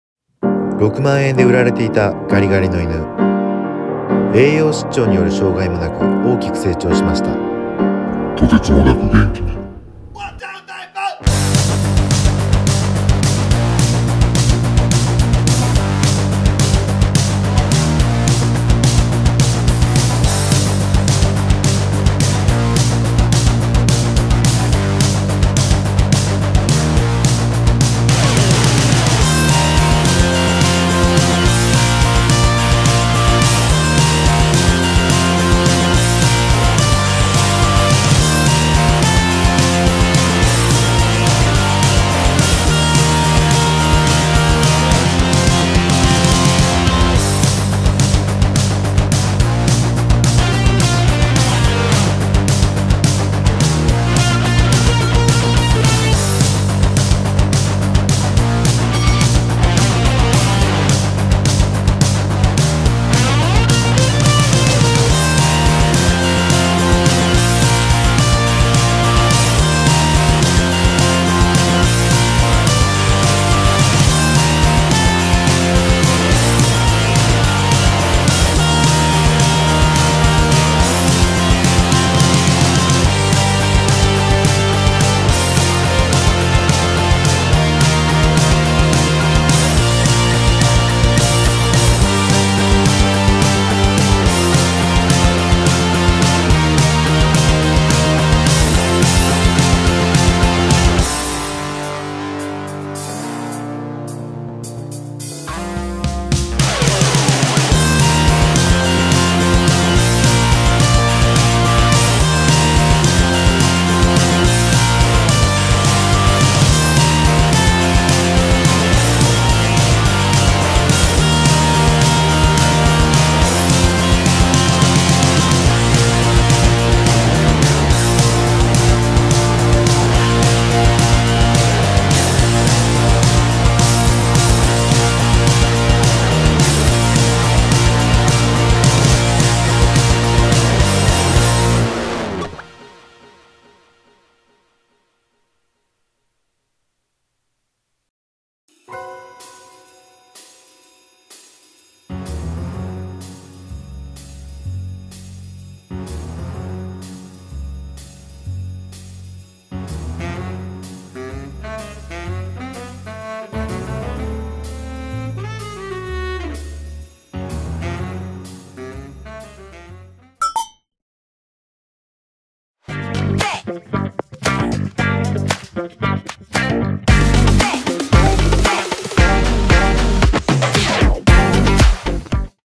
ボクの生きる道・・・ガリガリだった犬はくすくす成長してます！
売れ残りの6万円のガリガリの犬は、とてもいい子に成長しました

ペットショップの売れ残りで倉庫から現れたワイマラナーとゴールデンレトリバーの真っ黒なミックス犬

・ジビエフードの里山laboさん（偶然出会ったお姉さんたちのお店）